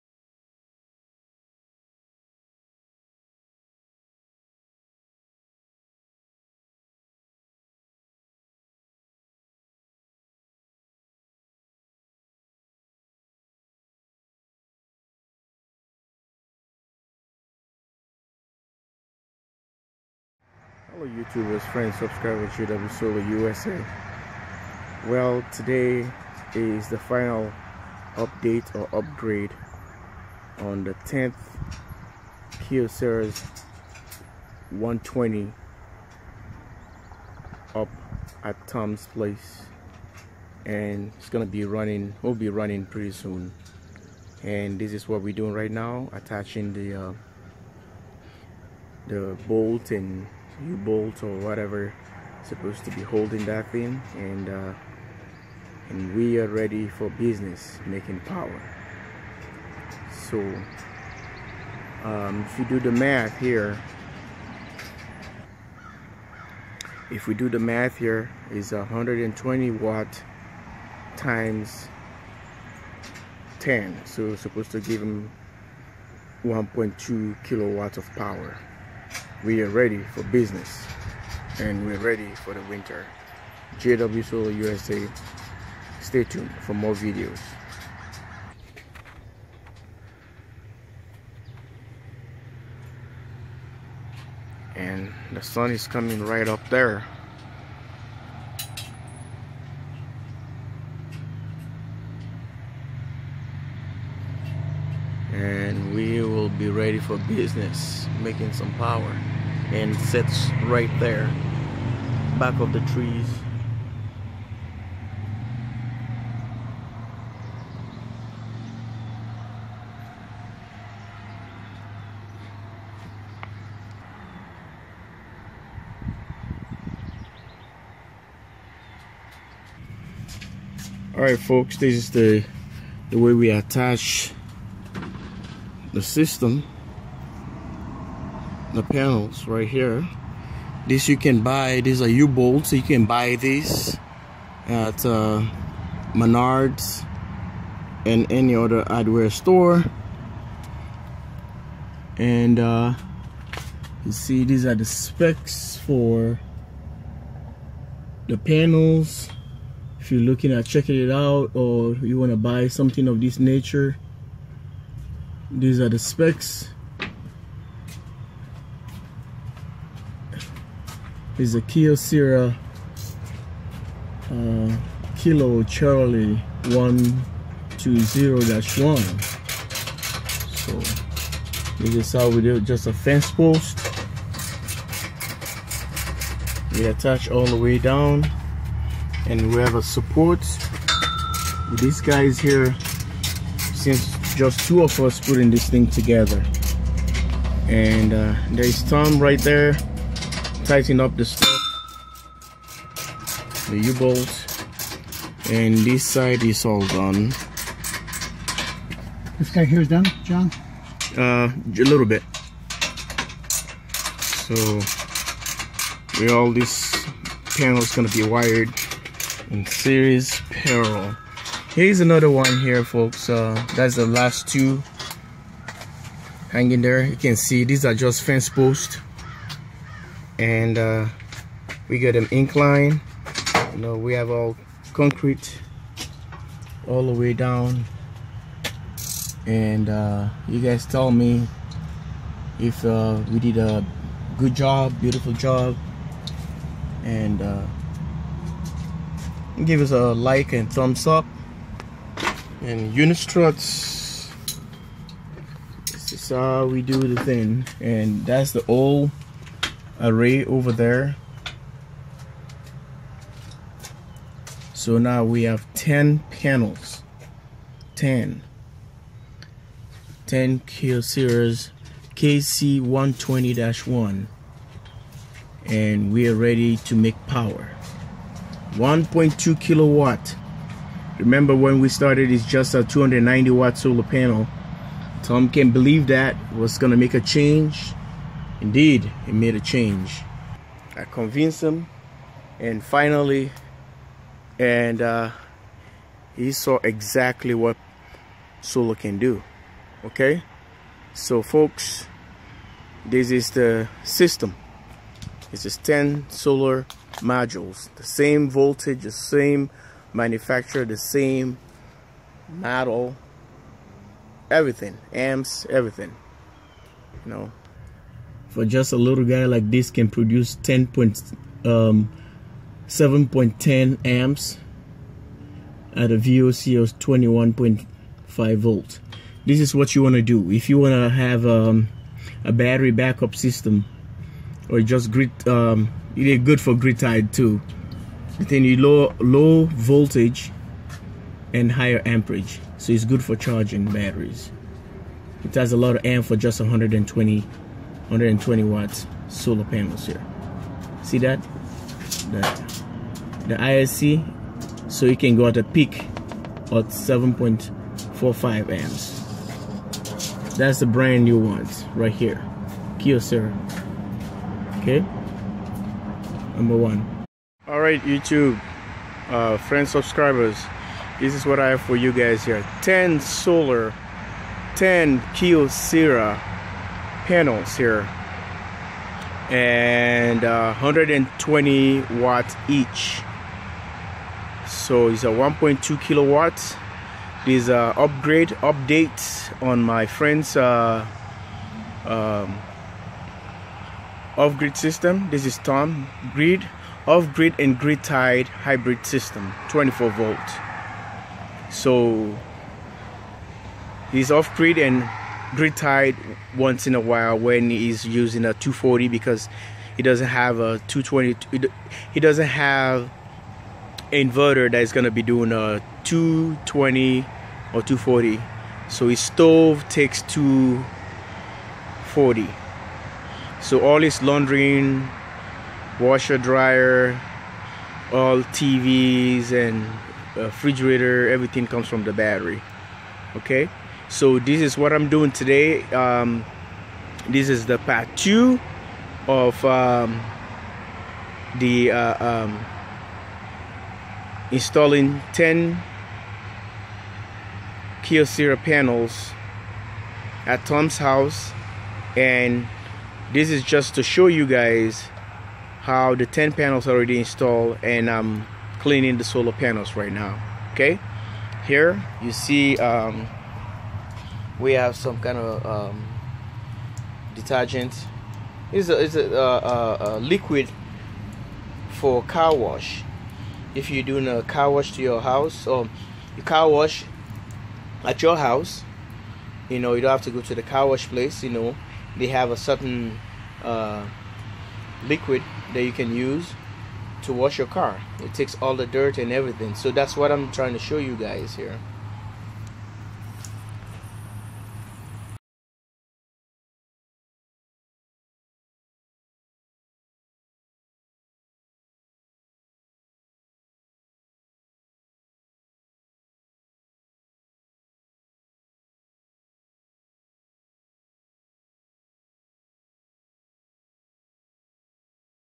SOLAR PANELS INSTALLATION: TEN KYOCERA KC120-1 SOLAR PANELS
Upgrading 10 KYOCERA KC120-1 Solar Panels  for my friend Tom.

STAY TUNE FOR MORE EPISODES OF THISINSTALLATION.